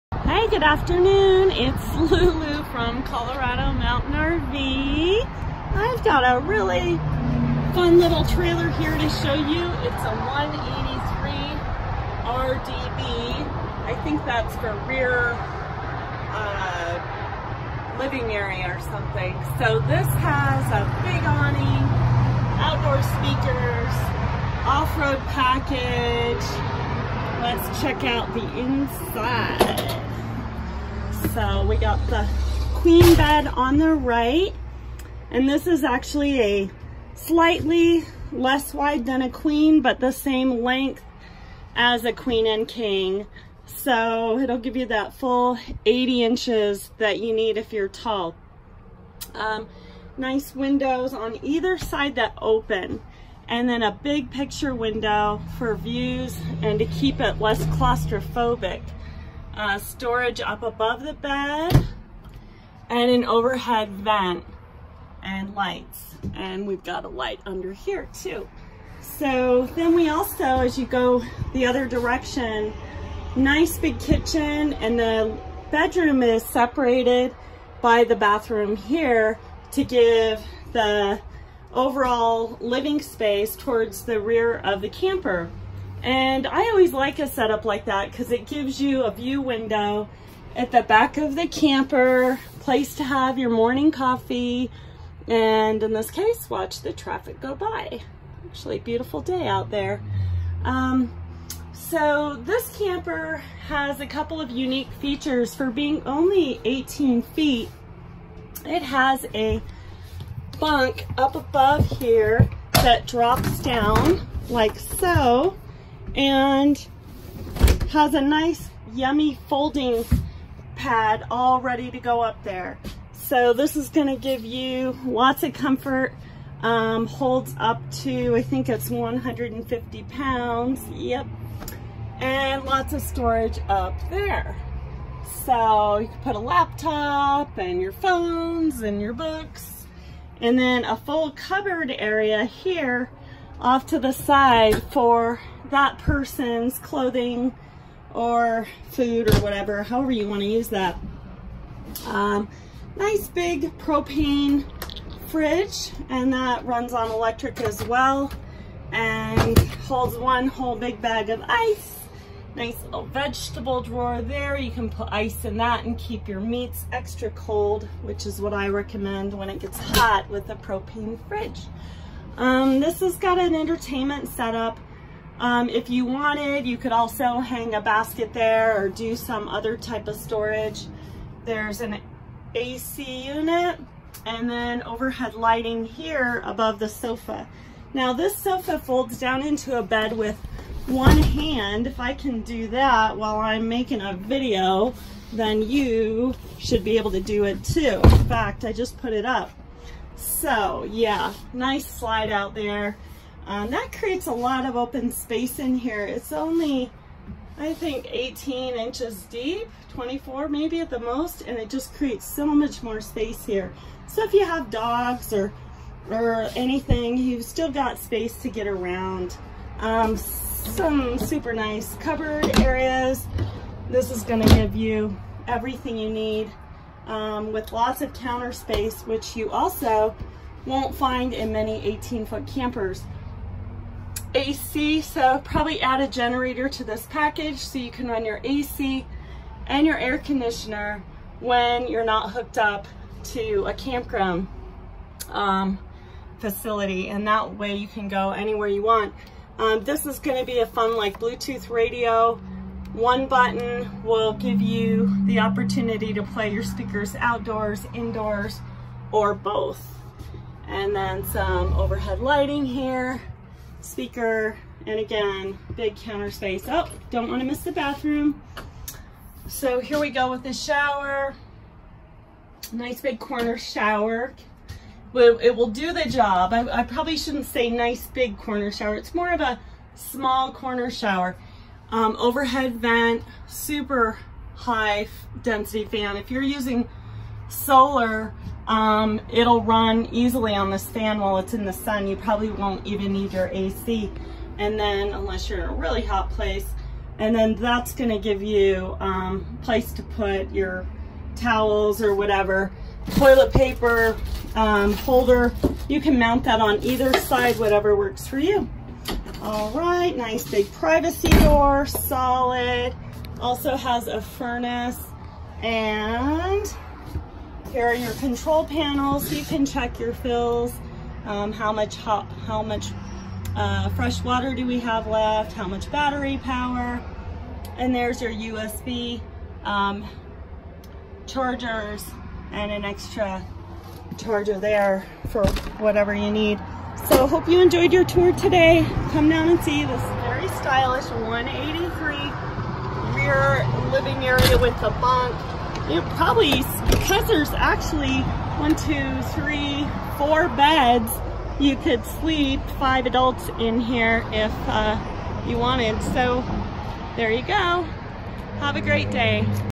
18’ Social 🥳Suite! Rear dining 🍧booth AND 🍿Sofa AND Bunk🍄
A Sofa, AND dinette Booth, AND Upper BUNK in only 18'!! Wow, that's something you don't find everywhere!! Nice front AND rear windows!! Double Bed, sleeps as many as 8 (littles ;-) or 5 adults!

Take a look at the manufactures website, full tour with specs about this unit here!